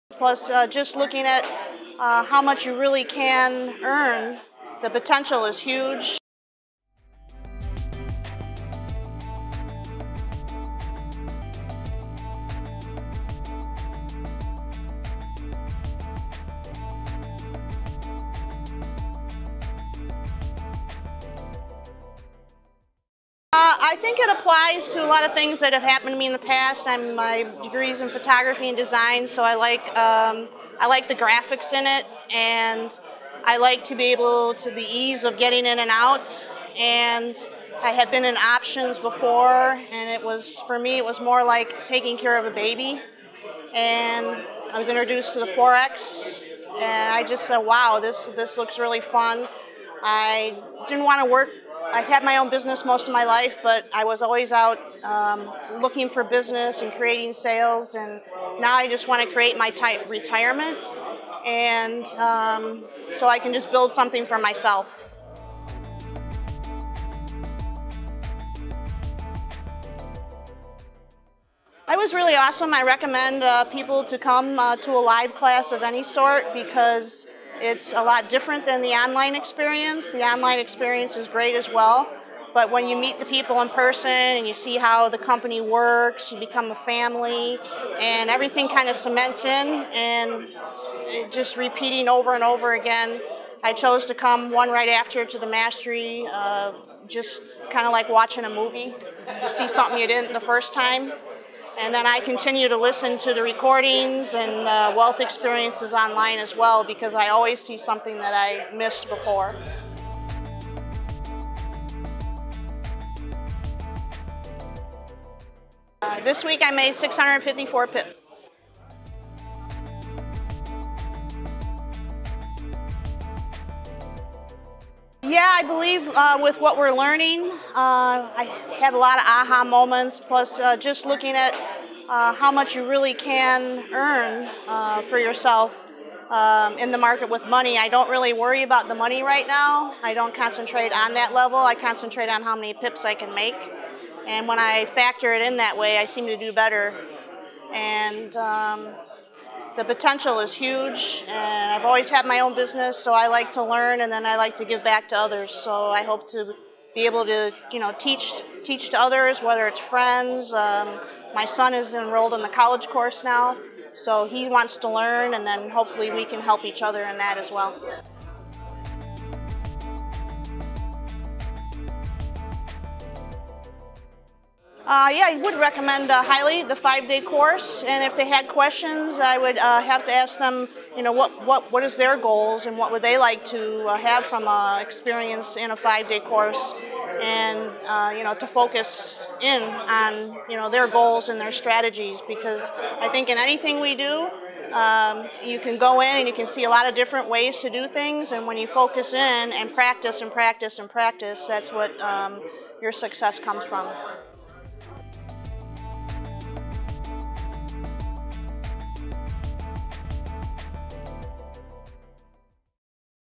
5-Day Forex Mastery Training Event Testimonial | Sharon F.
Sharon, a photographer and designer from Carlsbad, California describes her experience in the 5-Day Forex Mastery Training Event. She touches why she got into the Forex and how the courses taught by MTI have given her a few "aha" moments. She uses the metaphor of watching a movie to MTI's courses, "every time you come to a class or watch a recording, you see something you didn't see before."

To see more student success stories and learn more about Market Traders Institute,